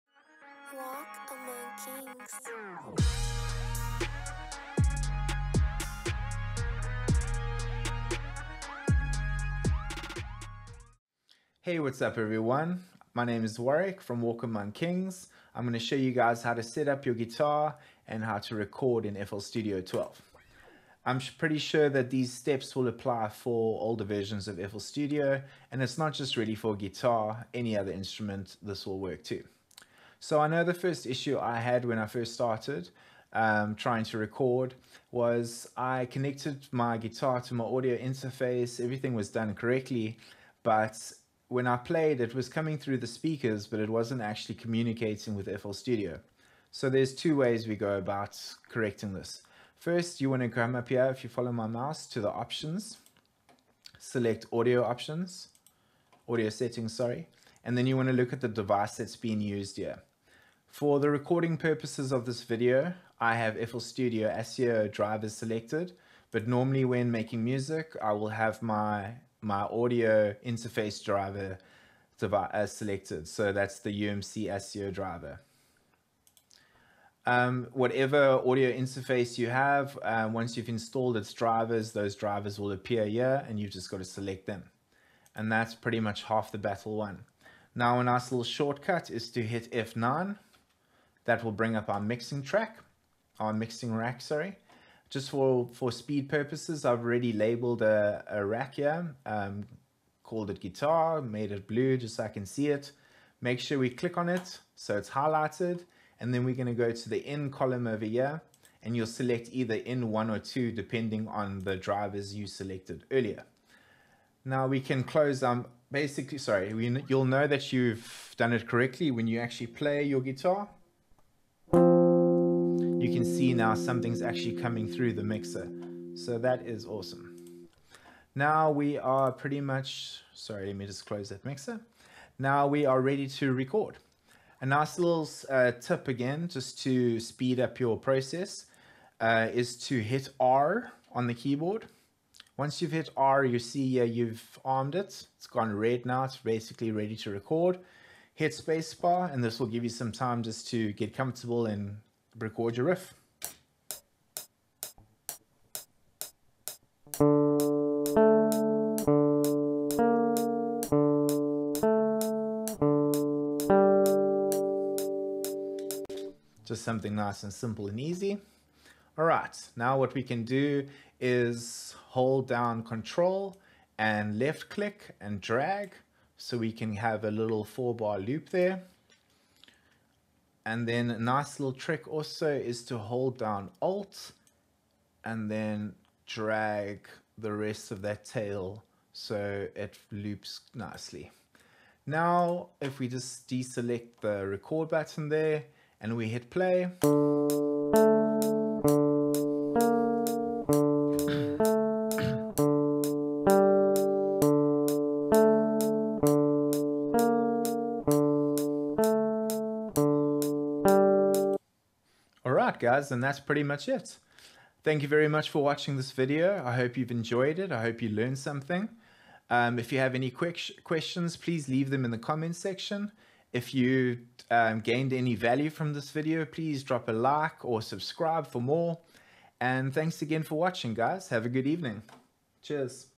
[Tutorial] How to Set up and Record your Guitar in FL Studio
In this Tutorial I show you how to set up and record your guitar or any other instrument in FL Studio 12.